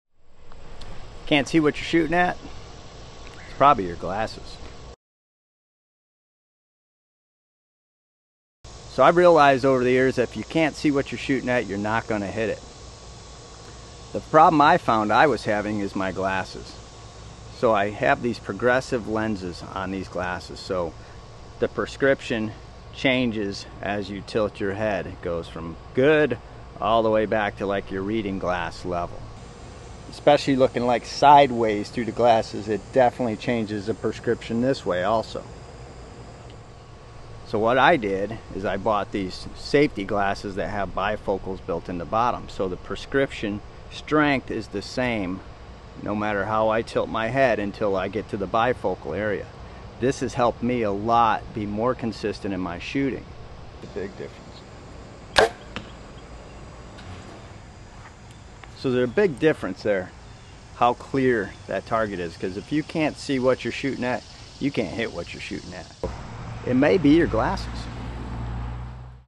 WHY CAN’T I SEE?? Progressive lenses, shooting Bow??
I found that I have a problem seeing with progressive lens glasses and sooting my bow.